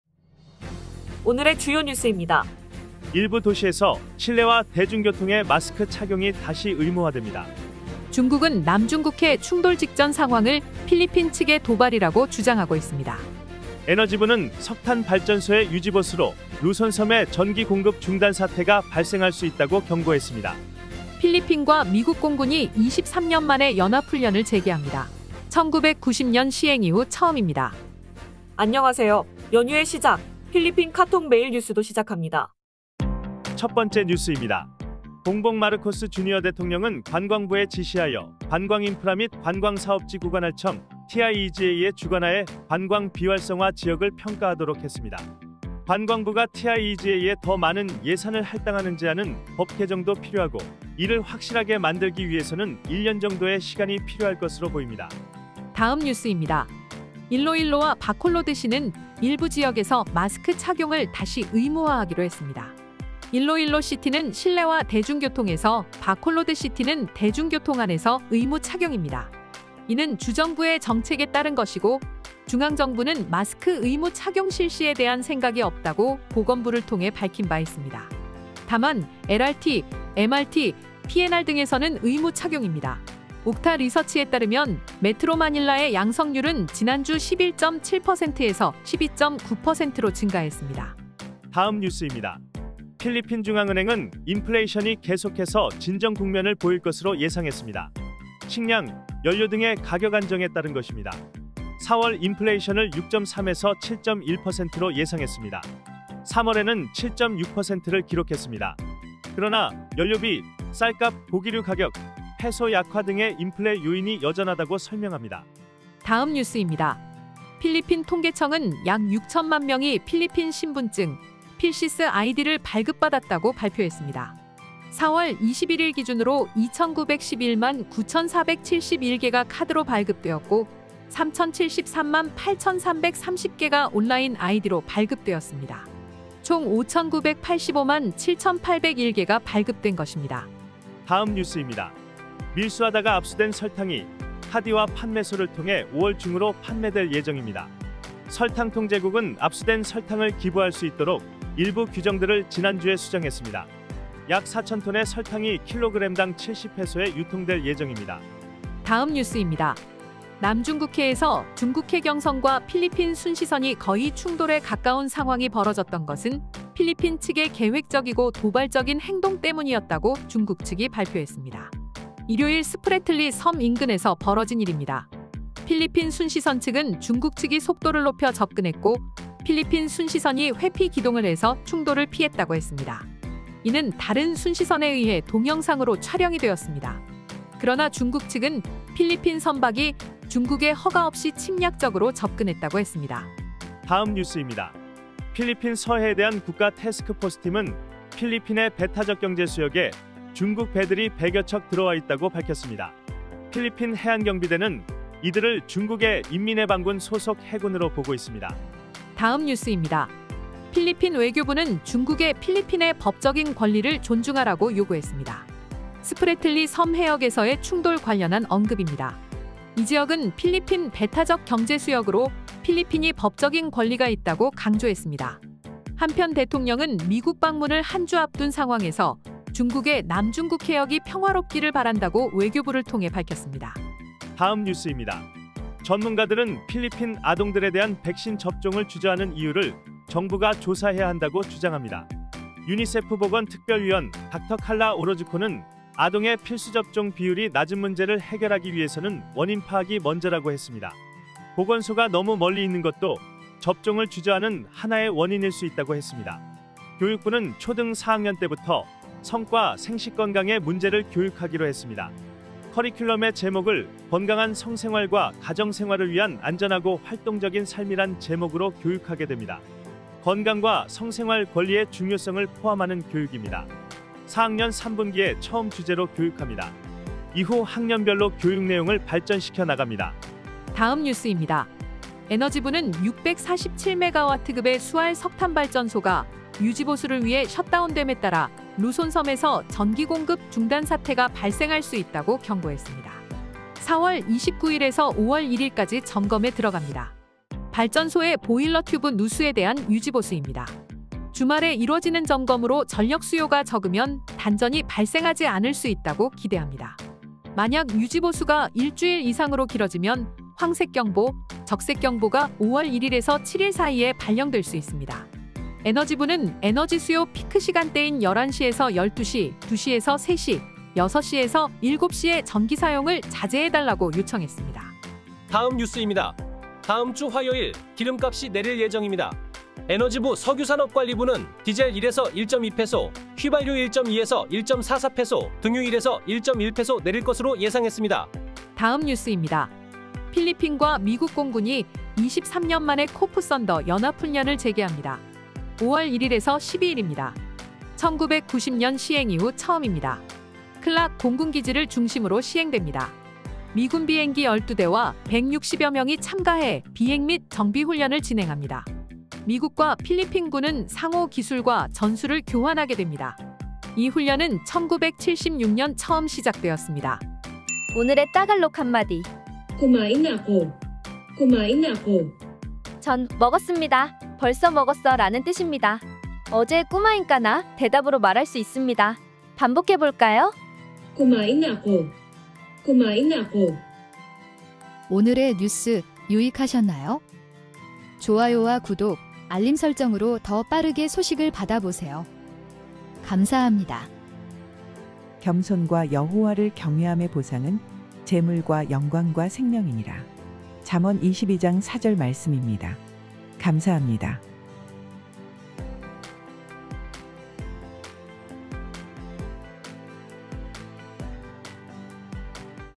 4월 29일 주요뉴스 | 필리핀 일부 도시 마스크 착용 다시 의무화 | 루손섬에 전기공급 중단사태 발생 경고 | 필리핀 미공군과 23년 만에 연합 훈련 재개 | 필리핀교민방송
2023년 4월 29일 필리핀유튜브 뉴스 헤드라인

1. 대통령, 관광 비활성화 지역 평가 지시

2. Iloilo와 Bacolod, 마스크 의무 착용

3. 필리핀 중앙은행, 인플레이션 진정 국면 보일 듯

4. 약 6천만 명 필리핀 신분증 발급받아

5. 밀수로 압수된 설탕, 5월 중으로 판매 예정

6. 중국, 남중국해 충돌상황 필리핀의 도발이라고 주장

7. 배타적 경제 수역에 중국 배 100여 척 들어와

8. 외교부, 중국에 필리핀의 법적 권리 존중 요구해

9. 아동 백신 접종 주저하는 이유 조사돼야

10. 석탄발전소 유지 보수로 셧다운 경고

11. 다음 주 화요일, 기름값 인하 예정

12. 필-미 공군, 23년 만에 연합 훈련 재개

오늘의 주요 뉴스입니다.

일부 도시에서 실내와 대중교통의 마스크 착용이 다시 의무화됩니다.

중국은 남중국해 충돌상황을 필리핀 측의 도발이라고 주장하고 있습니다.

에너지부는 석탄발전소의 유지보수로, 루손섬에 전기공급 중단사태가 발생할 수 있다고 경고했습니다.

필리핀과 미국 공군이 23년만에 연합훈련을 재개합니다. 1990년 시행이후 처음입니다.

안녕하세요, 연휴의 시작! 필리핀 카톡 매일 뉴스도 시작합니다.

첫번째 뉴스입니다. 봉봉 마르코스 주니어대통령은 관광부에 지시하여, 관광인프라 및 관광사업지구관할청, TIEZA의 주관하에 관광 비활성화 지역을 평가하도록 했습니다. 관광부가 TIEZA에 더 많은 예산을 할당하는 제안은, 법 개정도 필요하고 이를 확실하게 만들기 위해서는 1년정도의 시간이 필요할 것으로 보입니다.

다음 뉴스입니다. 일로일로와 바콜로드에 시는 일부 지역에서, 마스크 착용을 다시 의무화하기로 했습니다. 일로일로 시티는 실내와 대중교통에서, 바콜로드 시티는 대중교통안에서 의무 착용입니다. 이는 주정부의 정책에 따른 것이고, 중앙정부는 마스크 의무착용 실시에 대한 생각이 없다고, 보건부를 통해 밝힌 바 있습니다. 다만, LRT, MRT, PNR 등에서는 의무착용입니다. 옥타 리서치에 따르면, 메트로마닐라의 양성률은 지난주 11.7%에서 12.9%로 증가했습니다.

다음 뉴스입니다. 필리핀중앙은행은 인플레이션이 계속해서 진정국면을 보일 것으로 예상했습니다. 식량, 연료 등의 가격안정에 따른 것입니다. 4월 인플레이션을 6.3에서 7.1%로 예상했습니다. 3월에는 7.6%를 기록했습니다. 그러나 연료비, 쌀값, 고기류 가격, 페소 약화 등의 인플레요인이 여전하다고 설명합니다.

다음 뉴스입니다. 필리핀 통계청은 약 6천만명이 필리핀 신분증, 필시스 아이디를 발급받았다고 발표했습니다. 4월 21일 기준으로 이천구백십일만 9,471개가 카드로 발급되었고, 삼천칠십삼만 8,330개가 온라인 아이디로 발급되었습니다. 총 오천구백팔십오만 7,801개가 발급된 것입니다.

다음 뉴스입니다. 밀수하다가 압수된 설탕이, 카디와 판매소를 통해 5월 중으로 판매될 예정입니다. 설탕통제국은 압수된 설탕을 기부할 수 있도록, 일부 규정들을 지난주에 수정했습니다. 약 사천톤의 설탕이 킬로그램당 70페소에 유통될 예정입니다.

다음 뉴스입니다. 남중국해에서 중국해경선과 필리핀 순시선이 거의 충돌에 가까운 상황이 벌어졌던 것은, 필리핀 측의 계획적이고 도발적인 행동 때문이었다고 중국측이 발표했습니다. 일요일 스프래틀리 섬 인근에서 벌어진 일입니다. 필리핀 순시선측은 중국측이 속도를 높여 접근했고, 필리핀 순시선 회피 기동을 해서 충돌을 피했다고 했습니다. 이는 다른 순시선에 의해 동영상으로 촬영이 되었습니다. 그러나 중국측은, 필리핀 선박이 중국의 허가 없이 침략적으로 접근했다고 했습니다.

다음 뉴스입니다. 필리핀 서해에 대한 국가테스크포스팀은, 필리핀의 배타적경제 수역에 중국 배들이 100여척이 들어와있다고 밝혔습니다. 필리핀해안경비대는 이들을, 중국의 인민해방군 소속 해군으로 보고 있습니다.

다음 뉴스입니다. 필리핀 외교부는 중국에 필리핀의 법적인 권리를 존중하라고 요구했습니다. 스프래틀리 섬 해역에서의 충돌 관련한 언급입니다. 이 지역은 필리핀 배타적 경제수역으로, 필리핀이 법적인 권리가 있다고 강조했습니다. 한편 대통령은 미국 방문을 한주 앞둔 상황에서, 중국에 남중국해역이 평화롭기를 바란다고 외교부를 통해 밝혔습니다.

다음 뉴스입니다. 전문가들은 필리핀 아동들에 대한 백신 접종을 주저하는 이유를, 정부가 조사해야한다고 주장합니다. 유니세프 보건 특별위원 칼라 오로즈코는, 아동의 필수접종 비율이 낮은 문제를 해결하기 위해서는 원인파악이 먼저라고 했습니다. 보건소가 너무 멀리 있는 것도, 접종을 주저하는 한 원인일 수 있다고 했습니다. 교육부는 초등 4학년때부터, 성과 생식건강의 문제를 교육하기로 했습니다. 커리큘럼의 제목을 건강한 성생활과 가정생활을 위한 안전하고 활동적인 삶이란 제목으로 교육하게 됩니다. 건강과 셩생활 권리의 중요성을 포함하는 교육입니다. 4학년 3분기에 처음 주제로 교육합니다. 이후 학년별로 교육내용을 발전시켜 나갑니다.

다음 뉴스입니다. 에너지부는 647메가와트급의 수알 석탄발전소가, 유지보수를 위해 셧다운됨에 따라 루손섬에서 전기공급 중단사태가 발생할 수 있다고 경고했습니다. 4월 29일에서 5월 1일까지 점검에 들어갑니다. 발전소 보일러 튜브의 누수에 대한 유지보수입니다. 주말에 이뤄지는 점검으로 전력수요가 적으면, 단전이 발생하지 않을 수 있다고 기대합니다. 만약 유지보수가 일주일 이상으로 길어지면, 황색경보, 적색경보가 5월 이릴에서 7일 사이에 발령될 수 있습니다. 에너지부는 에너지수요 피크시간대인 11시에서 12시, 2시에서 3시, 6시에서 7시에 전기사용을 자제해달라고 요청했습니다.

다음 뉴스입니다. 다음주 화요일 기름값이 내릴 예정입니다. 에너지부 석유산업관리부는 디젤 1에서 1.2페소, 휘발유 1.2에서 1.44페소, 등유 1에서 1.1페소 내릴 것으로 예상했습니다.

다음 뉴스입니다. 필리핀과 미국 공군이 23년만에 코프 썬더 연합훈련을 재개합니다. 5월 1일에서 12일입니다. 1990년 시행이후 처음입니다. 클락 공군기지를 중심으로 시행됩니다. 미군 비행기 12대와 160여명이 참가해 비행 및 정비훈련을 진행합니다. 미국과 필리핀 군은 상호 기술과 전술을 교환하게 됩니다. 이 훈련은 1976년 처음 시작되었습니다.

겸손과 여호와를 경외함의 보상은, 재물과 영광과 생명이니라. 잠언 이십이장 4절 말씀입니다. 감사합니다.

공동제작 | CTS조이필리핀, 장재중

▶ 필리핀 카톡 매일뉴스는 월요일에서 토요일까지 매일 송출되는 필리핀 최초의 데일리 한인 교민 방송입니다.